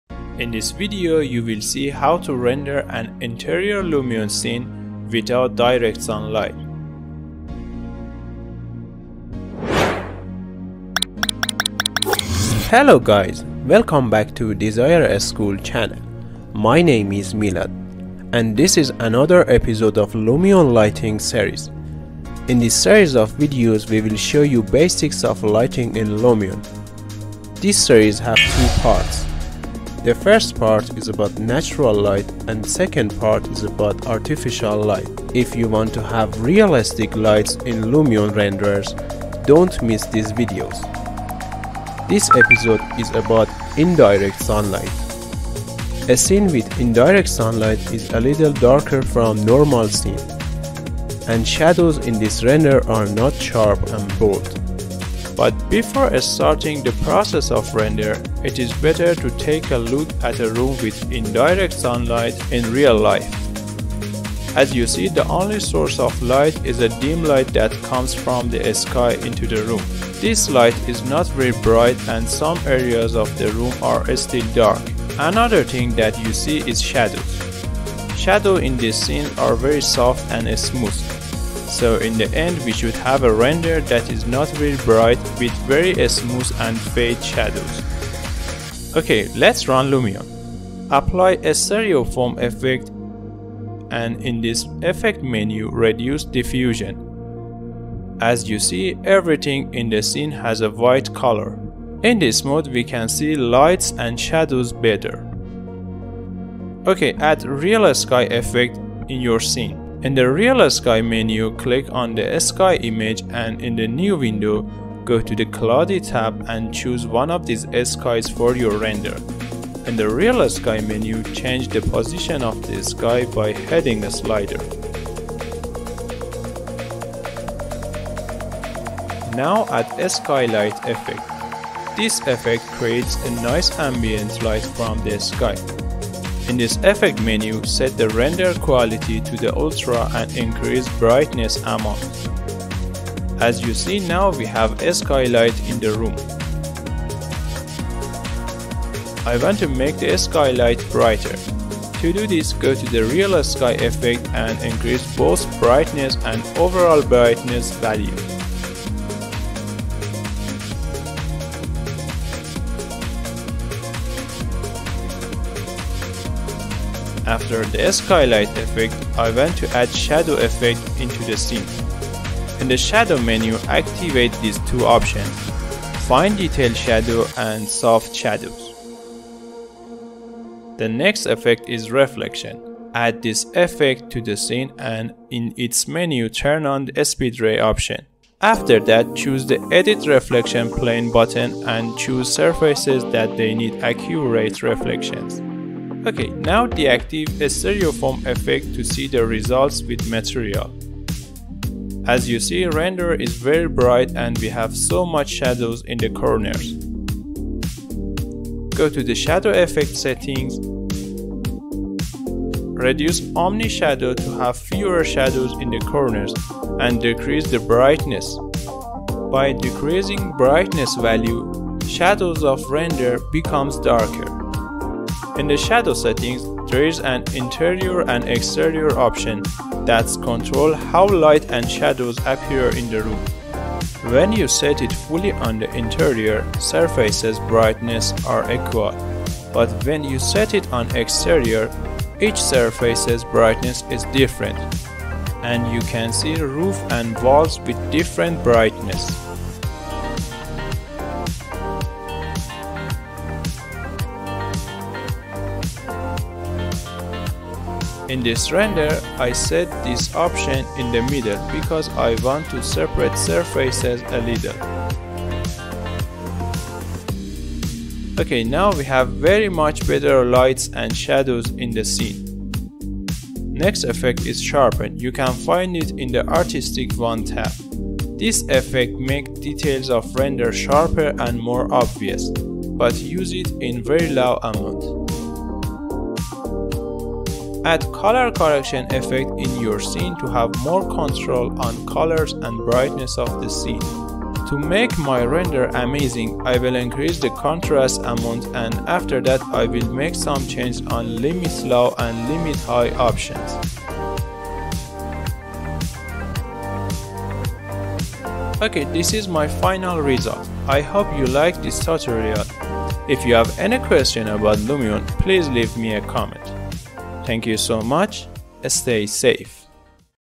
lumion interior render- lighting effects and settings tutorial (part 3-indirect sunlight)
hey guys
in this video i will show you effects and settings you need to create realistic lights in lumion Renders, watch these series of videos to learn lighting in lumion

we created a series of videos about lighting effects and settings in lumion
as you know lighting is very important to have realistic renders in lumion
so dont miss these videos

you can see previous episodes of lighting in lumion.